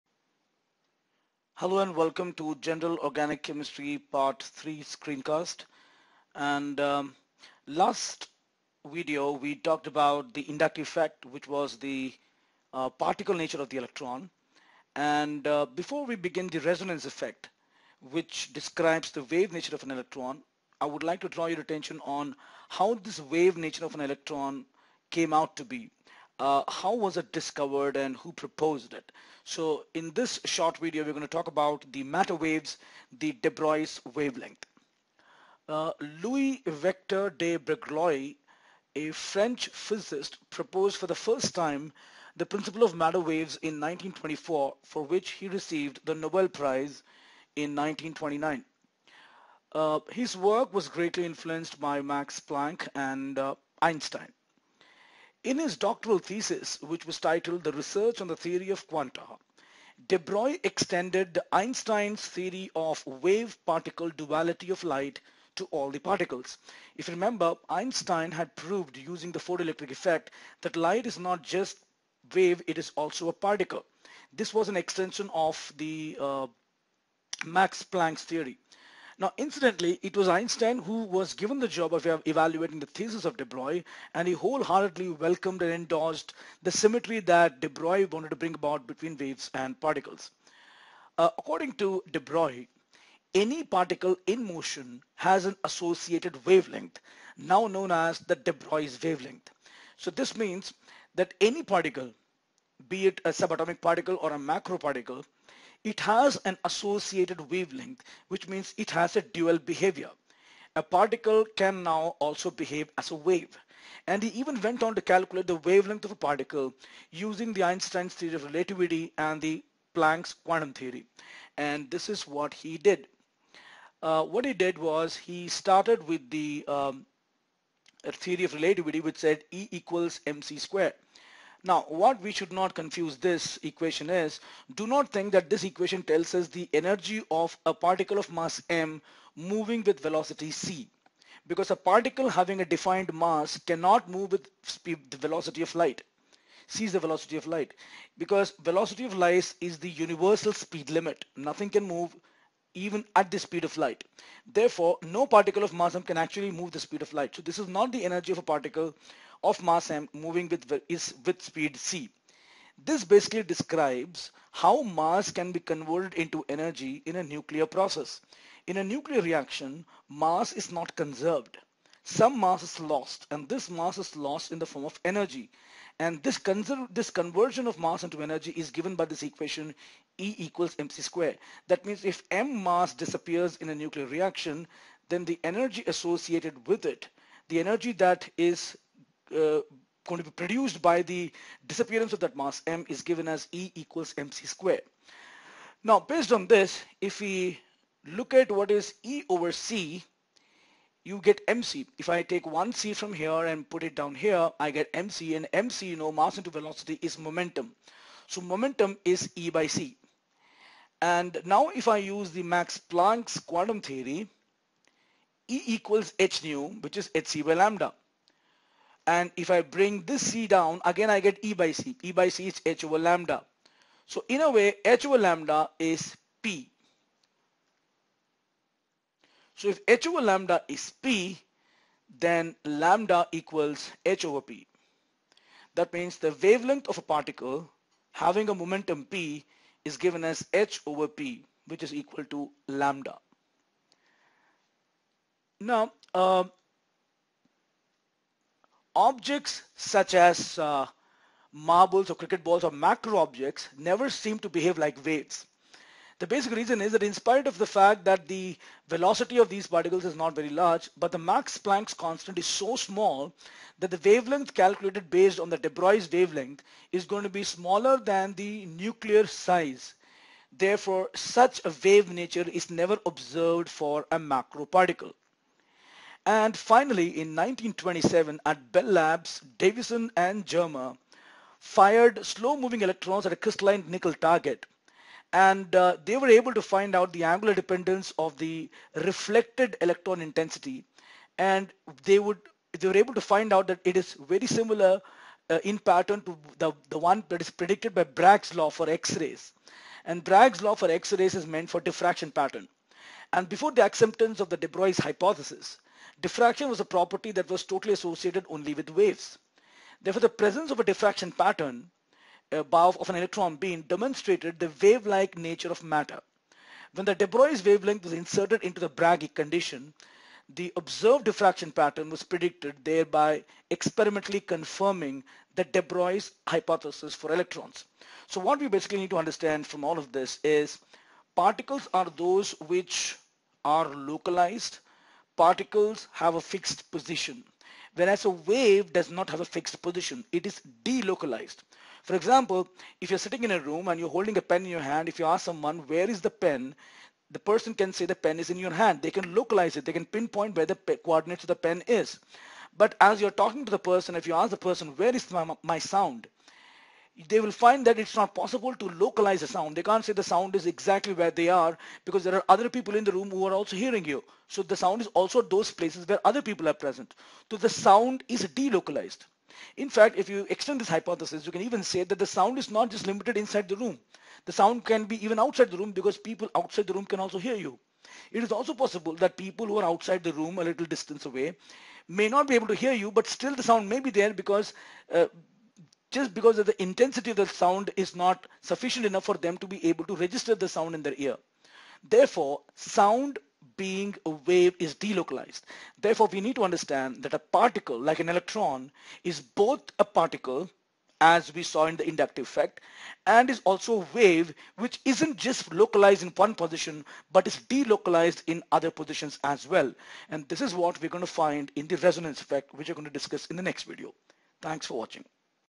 GOC-Part 3- Matter Waves-Precursor to Resonance Effect Video tutorial by C.V. Kalyan Kumar
Video Tutorial explaining the wave nature of matter.